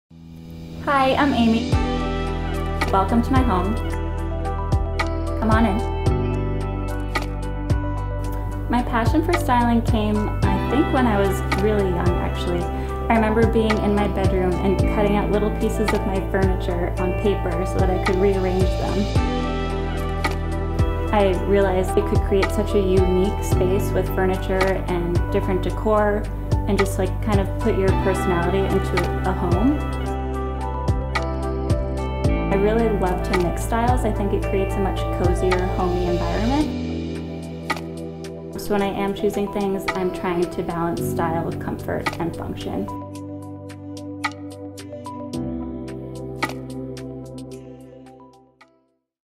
Home Tour with Stylist and Art Director Amy Pirri
Missy and I had the pleasure of photographing Stylist and Art Director Amy Pirri’s home in Central Massachusetts. Amy’s mission to “create a place where family and friends feel comfortable - literally and figuratively” is accomplished through lux and functional furniture pieces and decor. Amy’s philosophy when choosing pieces for the home is “I try to balance style, comfort and function. I choose things that, not only reflect personal style and feel, but also function well with our lifestyle. I want to truly live in my house and have guests feel like they can put their feet up, sit back and relax”.

Editorial featured in L'Essenzial